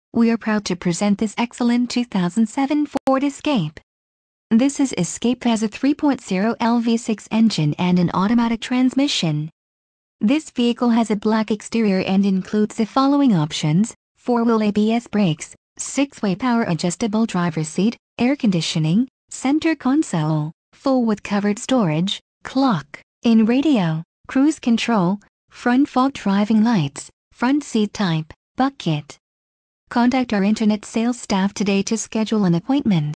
2007 Ford Escape Natchez Ford Lincoln Mercury
Natchez Ford Lincoln Mercury

2007 Ford Escape
Engine: 3.0L V6
Trans: Automatic 4-Speed
Color: Black
Mileage: 62,322

(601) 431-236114 Sgt. Prentiss Drive